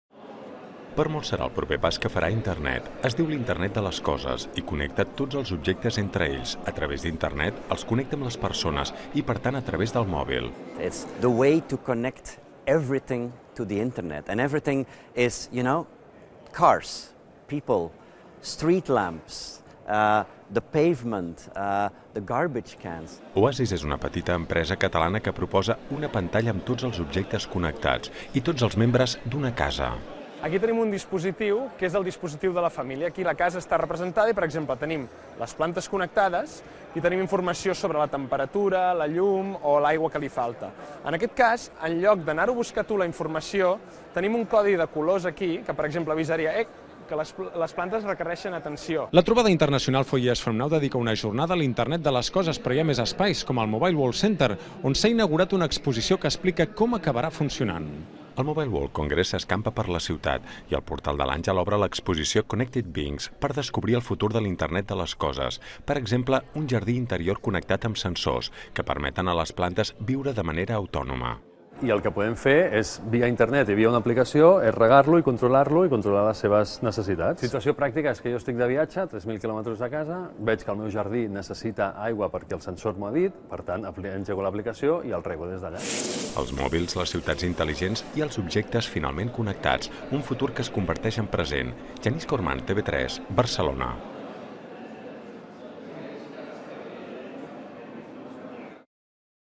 L'internet de les coses al Mobile World Congress i Mobile World Centre
Telenotícies vespre de TV3 del 03/03/2015 sobre la visita a l'exposició connectedbeings del Mobile World Centre, llençada durant el Mobile World Congress (març de 2015) i oberta fins al mes de juliol